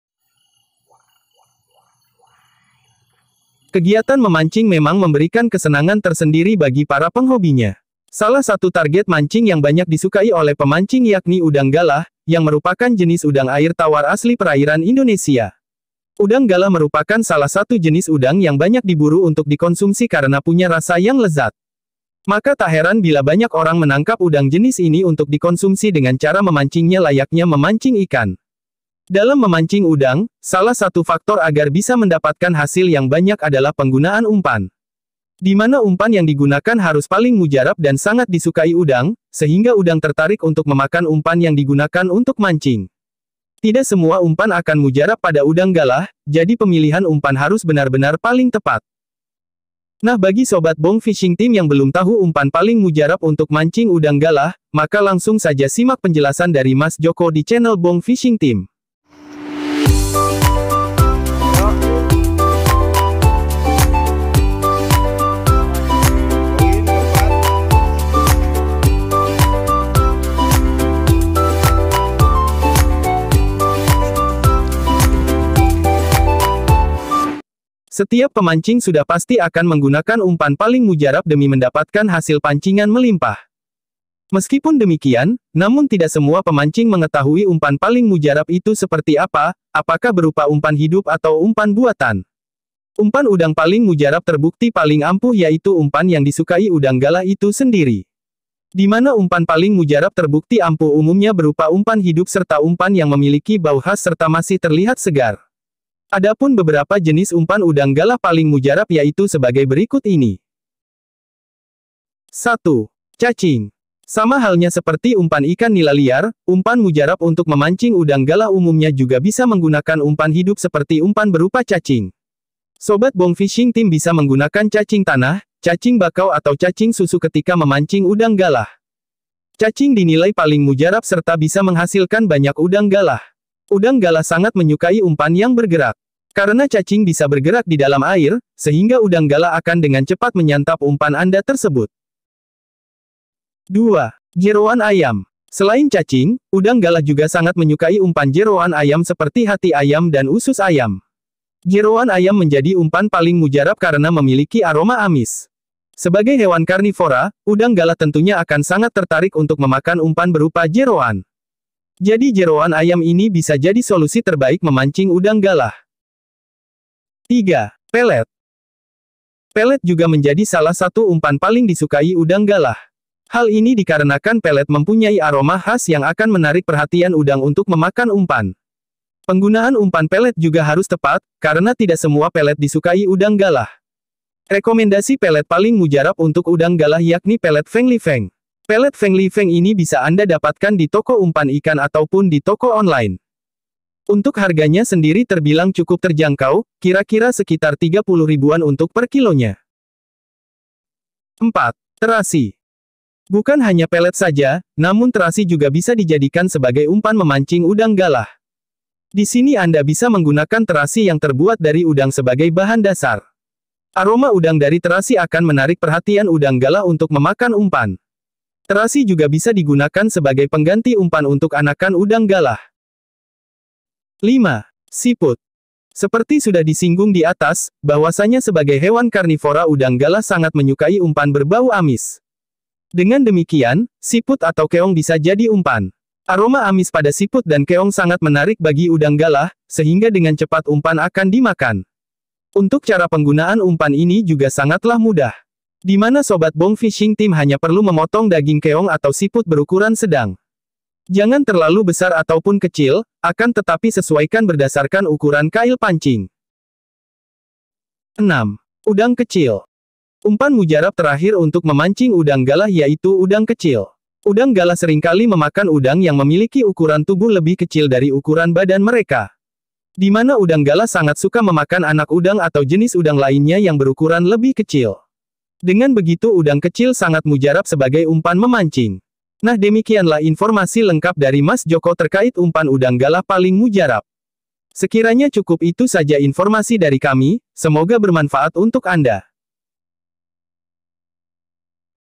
Rekomendasi Umpan Mancing Udang Galah Paling Mujarab 100% Ampuh
Udang galah merupakan salah satu jenis udang yang banyak diburu untuk dikonsumsi karena punya rasa yang lezat. Maka tak heran bila banyak orang menangkap udang jenis ini untuk dikonsumsi dengan cara memancingnya layaknya memancing ikan.
Serial                   :  Udang Galah
Episode               : Umpan Mancing